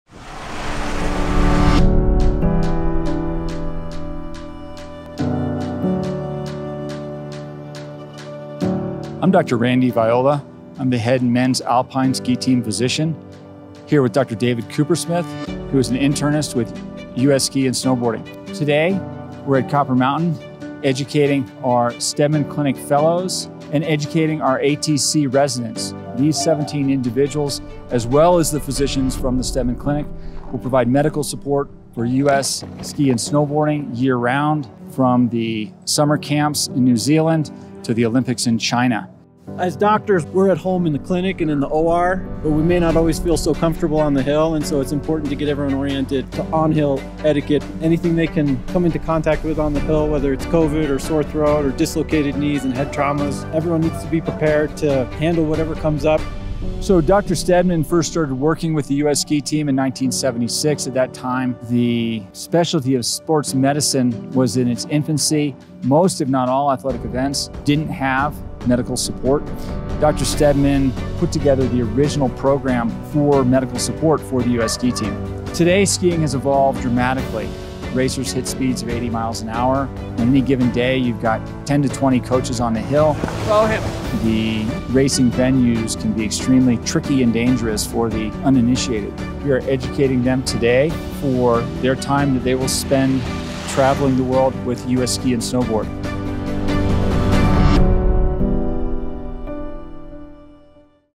On-Hill Orientation for Fellows and ATCs
The Steadman Clinic Sports Medicine Fellows and Athletic Training Residents provide medical support for the US Ski and Snowboard Teams all year long. Here is a look at a day of on-hill training to get them ready to travel the world with US Ski and Snowboard.